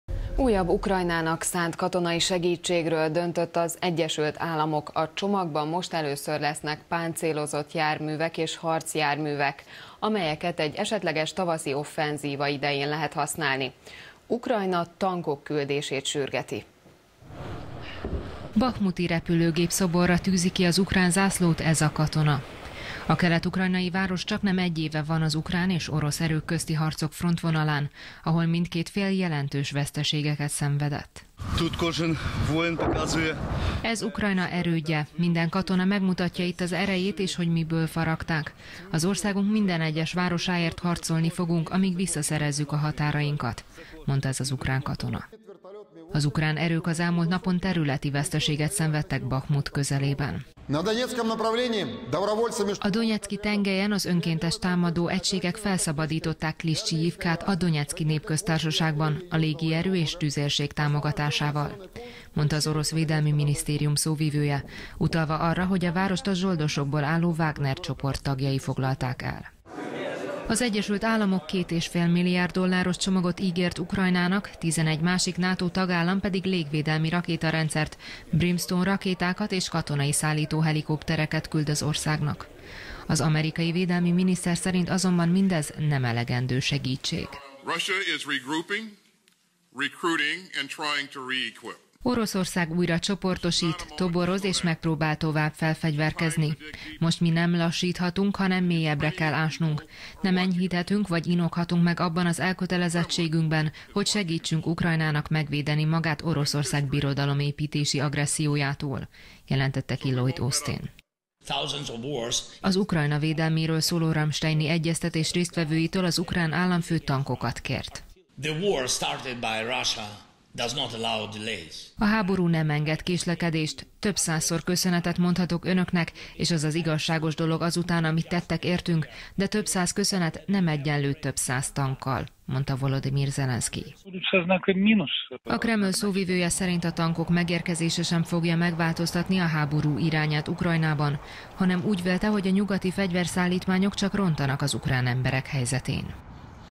Az orosz Wagner-csoport taktikai előretörést hajtott végre a Donbászban
Újabb Ukrajnának szánt katonai segítségről döntött az Egyesült Államok. A csomagban most először lesznek páncélozott járművek és harcjárművek, amelyeket egy esetleges tavaszi offenzíva idején lehet használni. Ukrajna tankok küldését sürgeti.